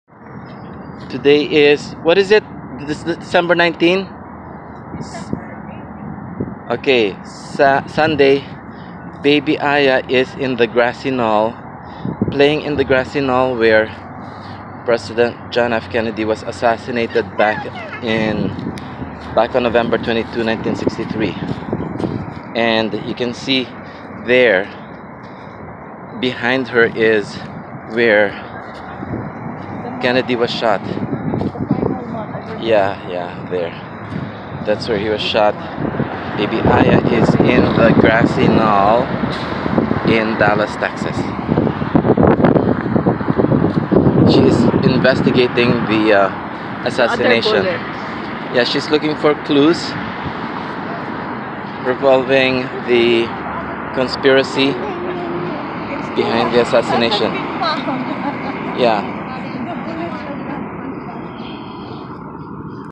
Baby Aya on Grassy Knoll in Dallas
Baby Aya on Grassy Knoll in Dallas where JFK was killed, on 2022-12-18